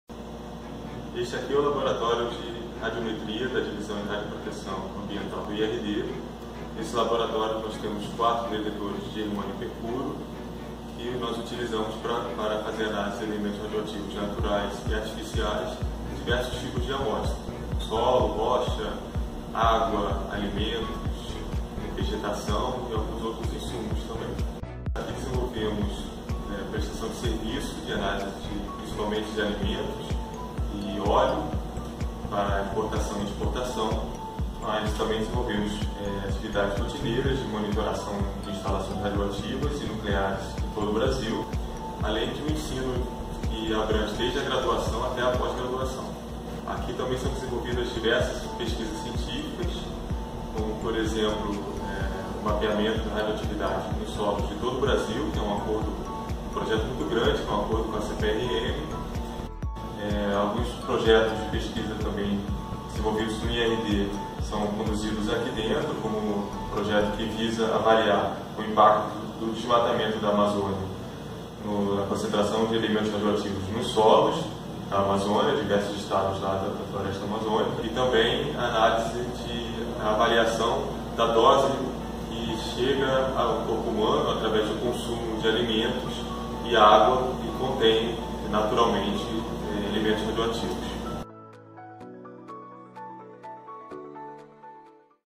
Laboratório de Radiometria IRD
No Laboratório de Radiometria do IRD são realizadas análises de radionuclídeos em amostras ambientais e em produtos e insumos importados e exportados pelo país. São realizadas pesquisas, serviços tecnológicos e assessoria técnico-científica no âmbito da Divisão de Radioproteção do IRD. Sempre buscando a excelência na formação de profissionais e pesquisadores, desde estudantes de iniciação científica até o pós-doutorado.